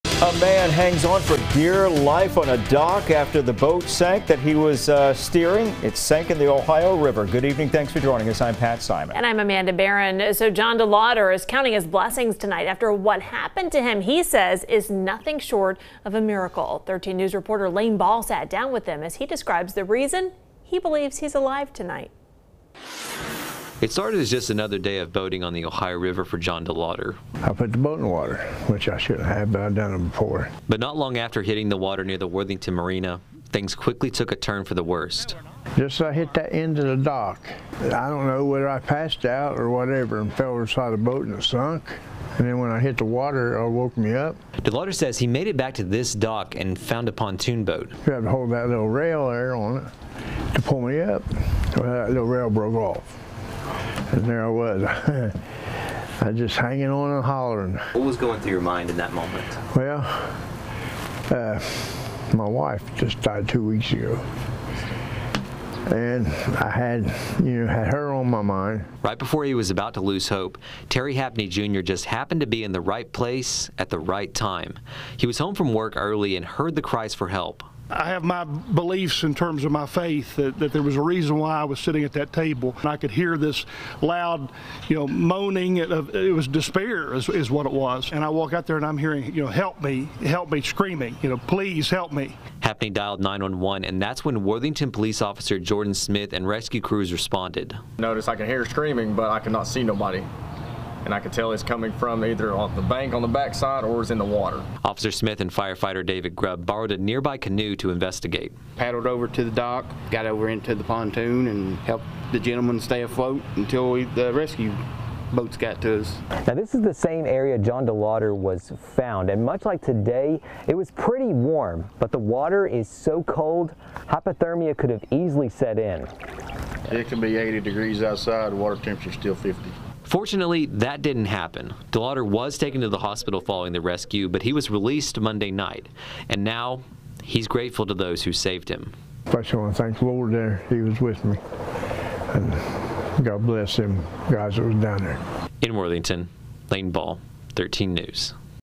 Man rescued from Ohio River in Kentucky speaks out
It was a scary situation for one man when his boat sank in the Ohio River and he was left hanging onto a boat dock for dear life for more than an hour.